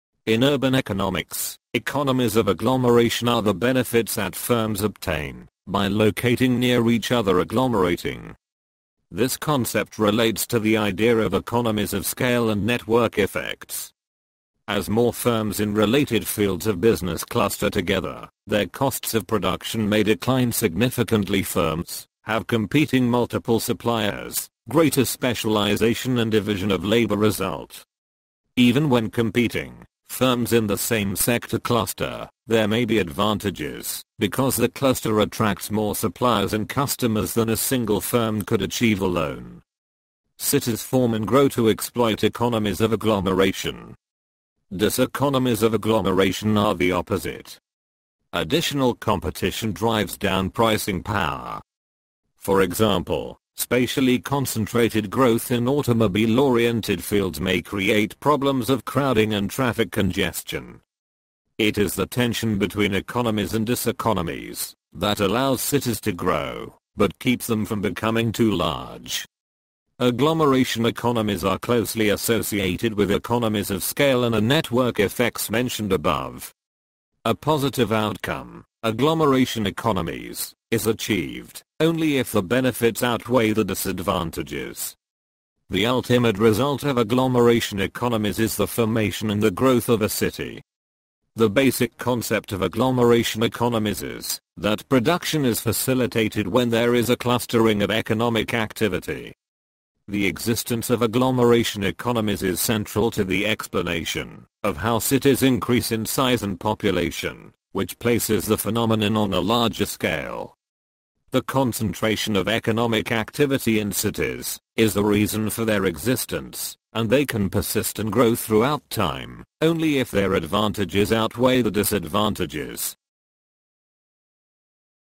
Economies of agglomeration
In urban economics, economies of agglomeration are the benefits that firms obtain by locating near each other ('agglomerating'). This concept relates to the idea of economies of scale and network effects. As more firms in related fields of business cluster together, their costs of production may decline significantly (firms have competing multiple suppliers; greater specialization and division of labor result). Even when competing firms in the same sector cluster, there may be advantages because the cluster attracts more suppliers and customers than a single firm could achieve alone. Cities form and grow to exploit economies of agglomeration.

Diseconomies of agglomeration are the opposite. Additional competition drives down pricing power. For example, spatially concentrated growth in automobile-oriented fields may create problems of crowding and traffic congestion. It is the tension between economies and diseconomies that allows cities to grow but keeps them from becoming too large.

Agglomeration economies are closely associated with economies of scale and the network effects mentioned above. A positive outcome, agglomeration economies, is achieved only if the benefits outweigh the disadvantages. The ultimate result of agglomeration economies is the formation and the growth of a city.

The basic concept of agglomeration economies is that production is facilitated when there is a clustering of economic activity. The existence of agglomeration economies is central to the explanation of how cities increase in size and population, which places the phenomenon on a larger scale. The concentration of economic activity in cities is the reason for their existence, and they can persist and grow throughout time only if their advantages outweigh the disadvantages.